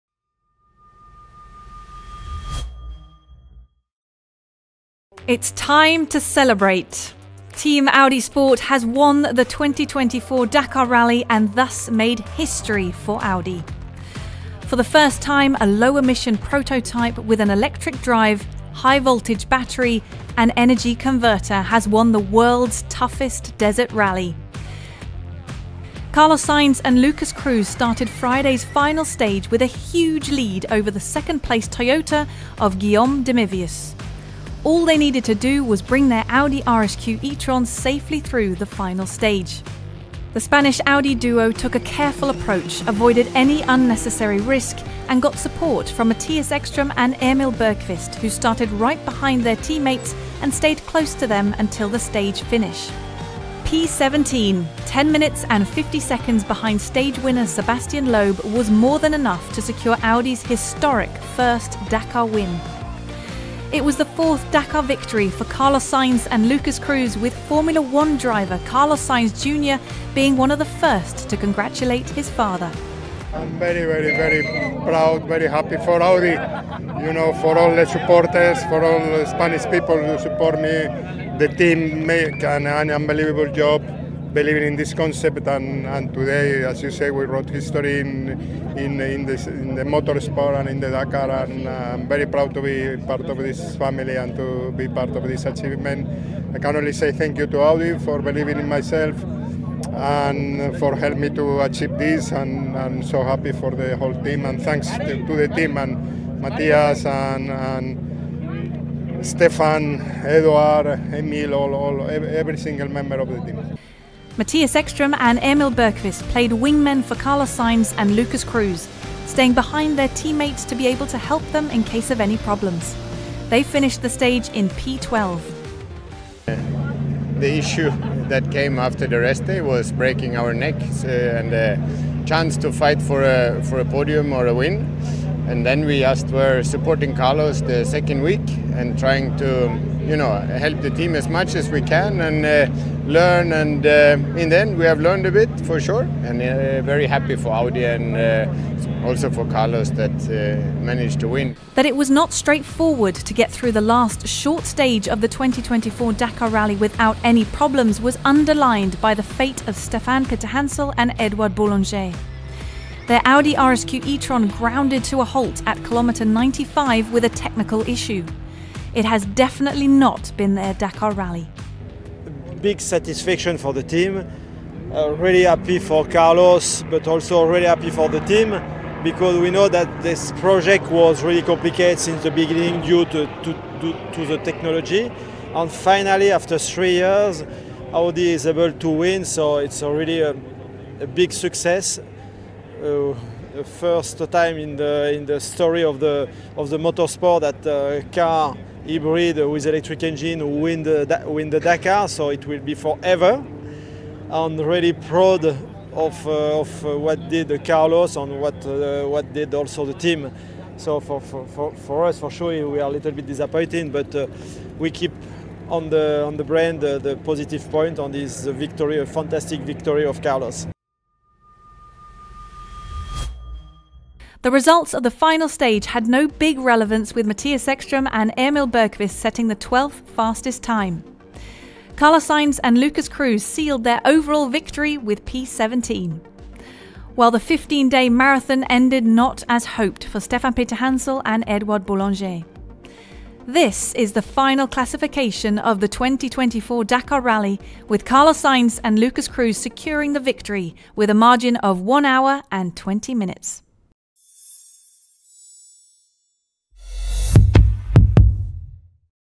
JOUR 15 - INSIDE DAKAR RALLY 2024 | Team Audi Sport
Lors de la douzième étape du Rallye Dakar, Carlos Sainz & Lucas Cruz ont remporté leur quatrième victoire au classement général du Rallye Dakar et la première pour Audi. Ils ont terminé 80 minutes devant Guillaume de Mevius & Xavier Panseri, avec la troisième place revenant à Sébastien Loeb & Fabian Lurquin